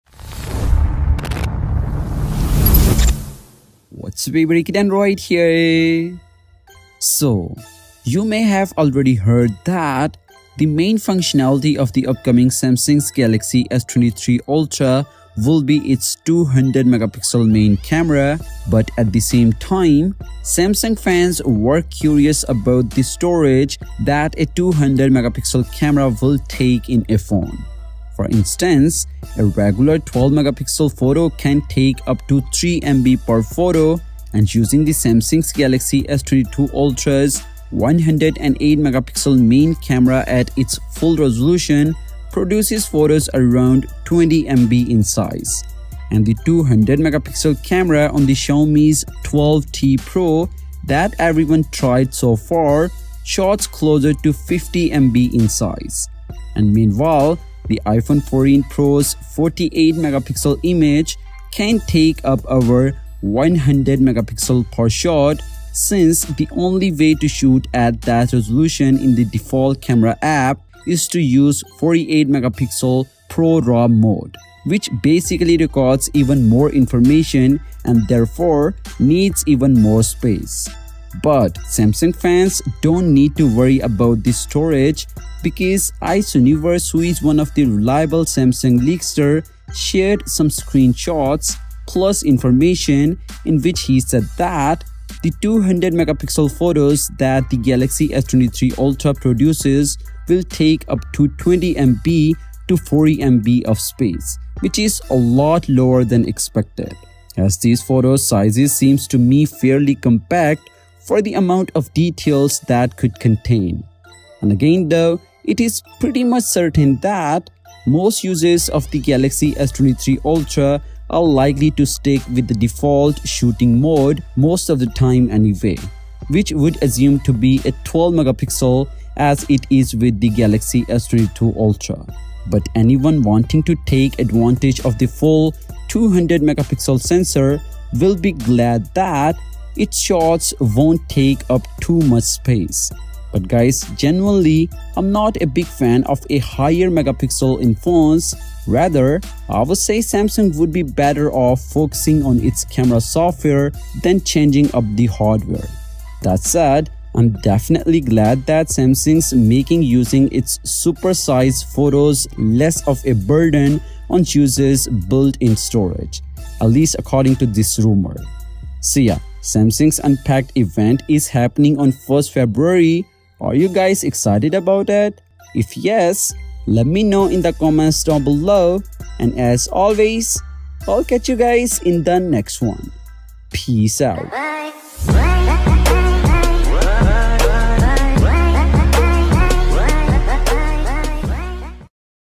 Samsung Galaxy S23 Ultra - YES, This Is GREAT!!
Hey guys..
So Samsung Galaxy S23 ultras 200MP Camera will not take too much of the storage in your phone which is a great news for Samsung Fans!!